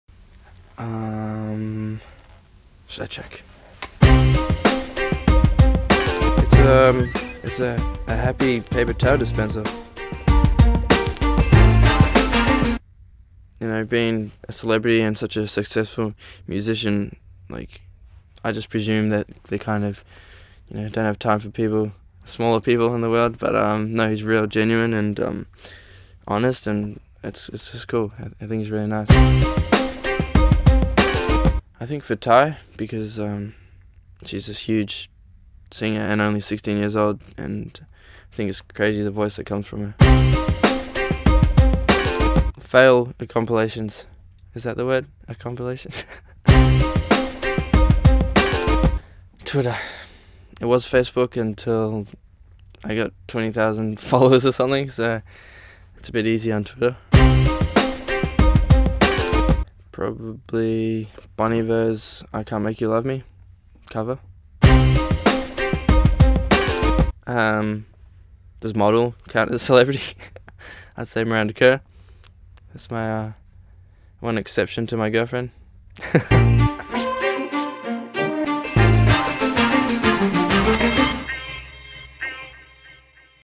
Get to know Lakyn Heperi from The Voice
Lakyn Heperi from Channel Nine's The Voice sits down with Nova FM. What makes this 19-year-old semi-pro skateboarder tick? What was the last photo he took on his phone? What has he learnt about Joel? Lakyn spills the beans